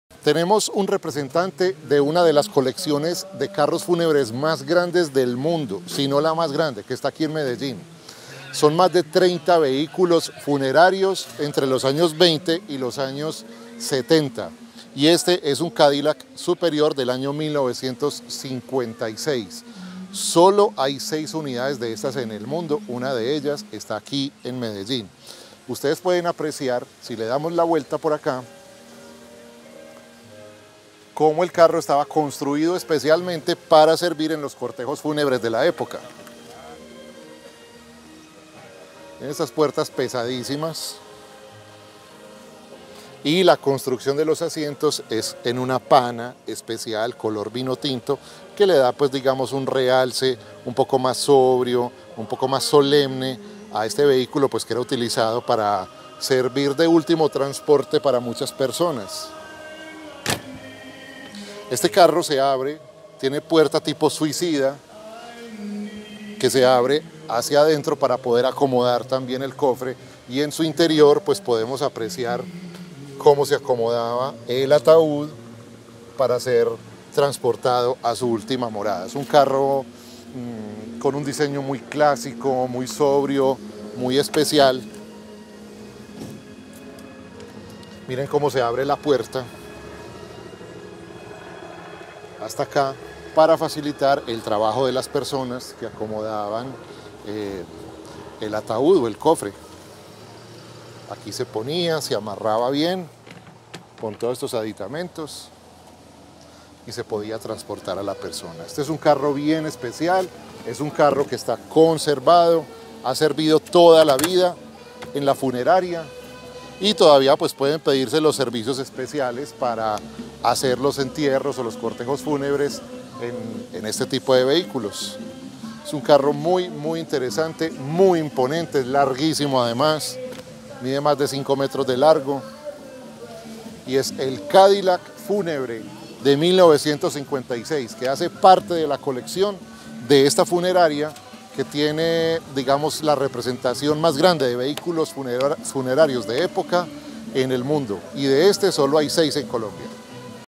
#1956 - CONCOURS D´ELEGANCE COLOMBIA #2018
Una de las piezas más escasas de la inmensa colección de la Funeraria San Vicente Medellín, una de las más grandes recopilaciones de coches fúnebres en el mundo.